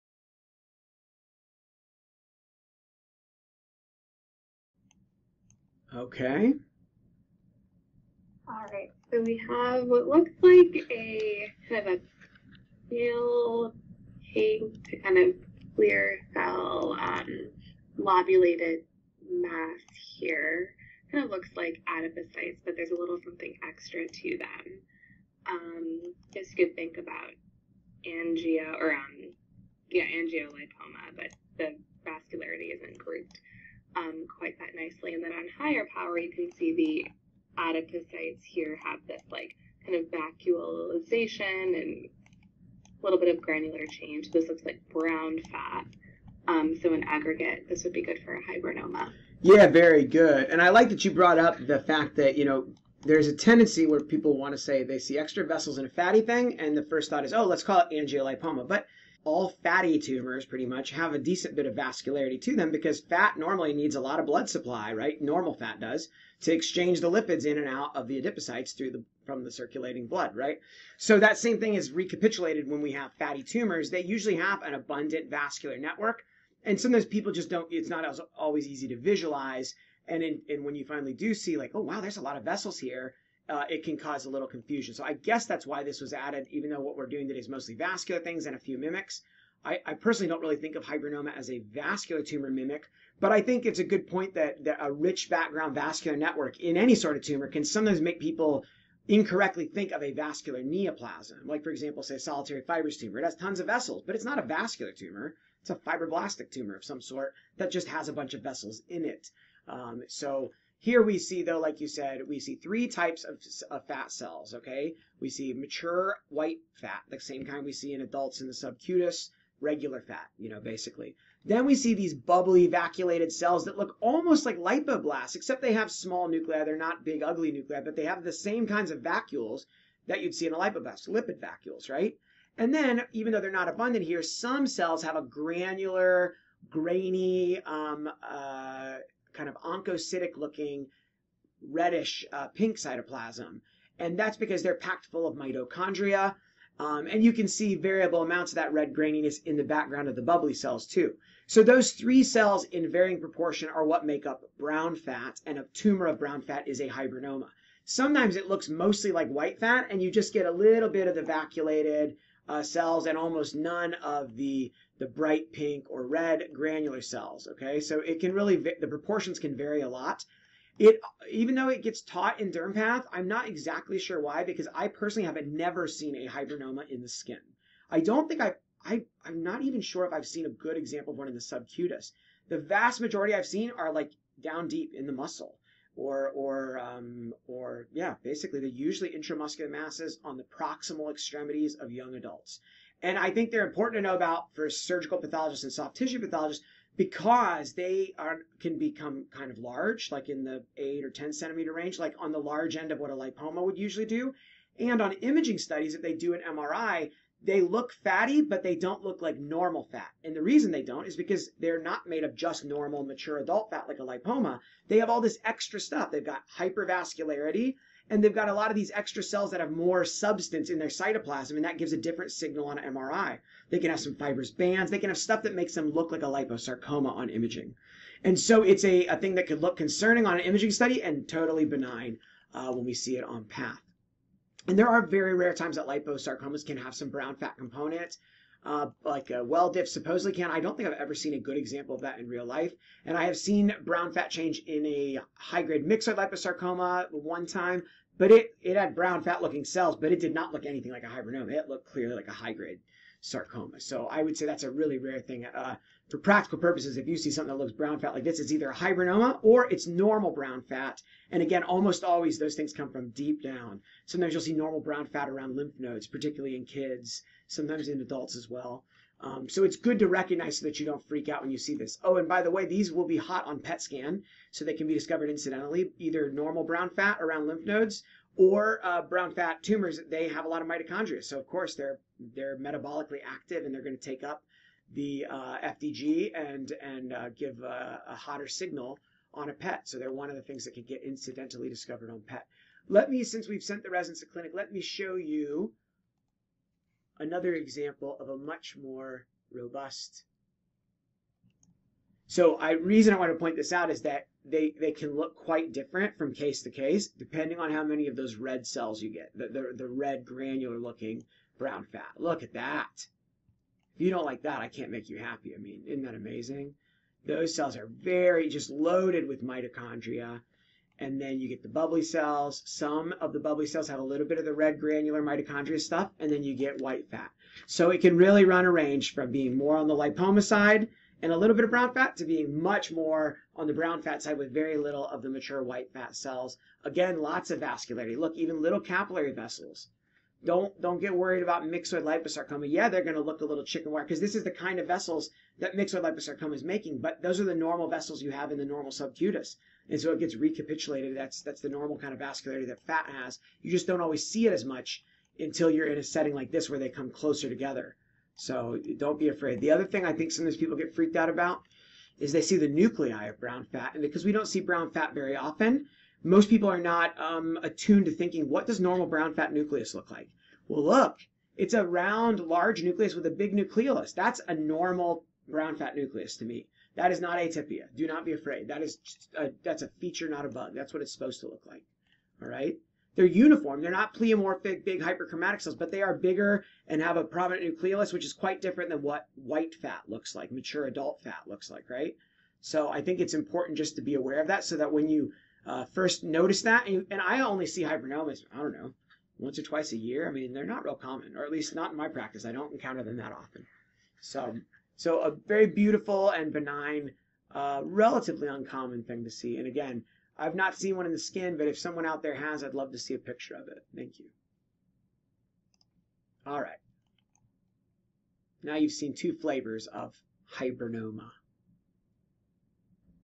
Hibernoma (benign brown fat tumor) pathology dermpath dermatology dermatopathology
Excerpt from my vascular tumors of skin board review with my residents and fellows. 31 cases in 3 hours. Enjoy!